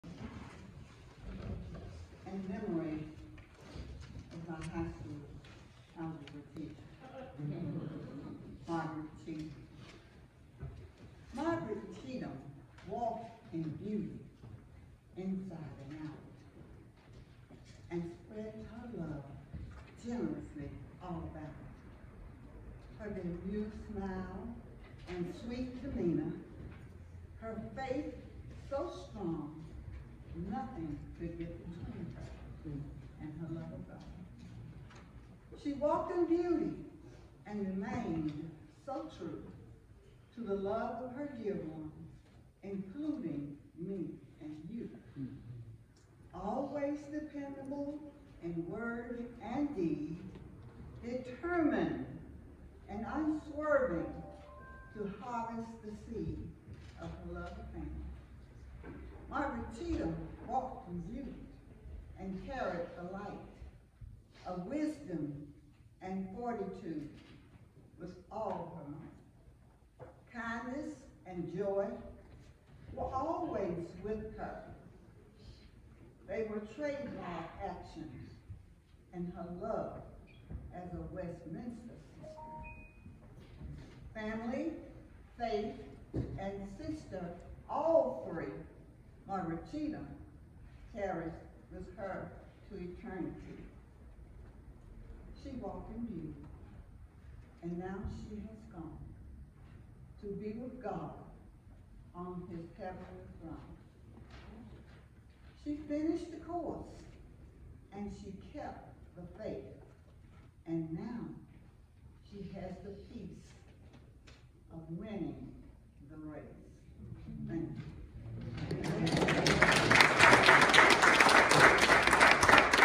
Tribute to Mrs. Margaret E. Cheatham
A Poem by Mrs. Gloria A. Dennard
Former Student of Mrs. Cheatham

Westminster Presbyterian Church
20 Sixth Avenue SW